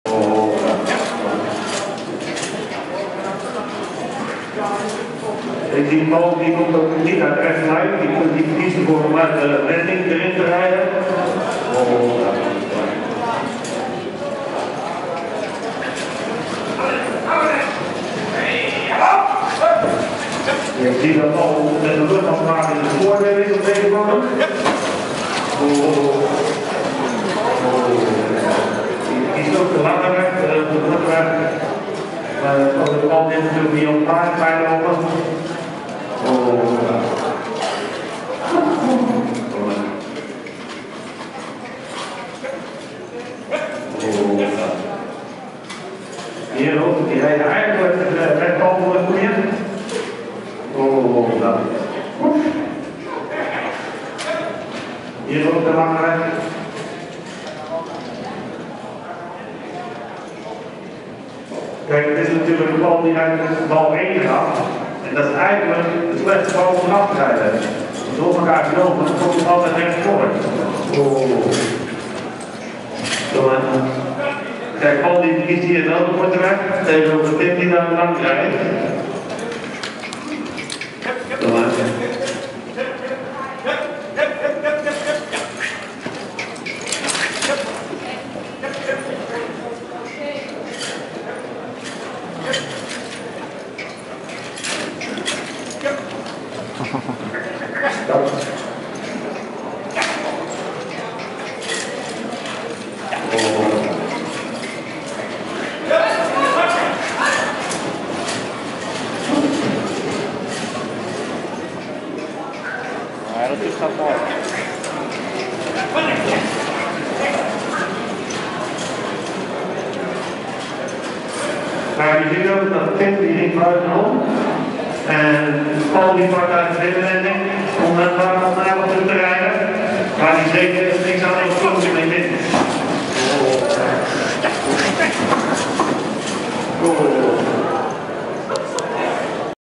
Nimmerdor marathon instructie Theo Timmermans - Paul Loman
Instructie marathon aan Paul Loman door Marathonrijder Theo Timmermans. Complete ronde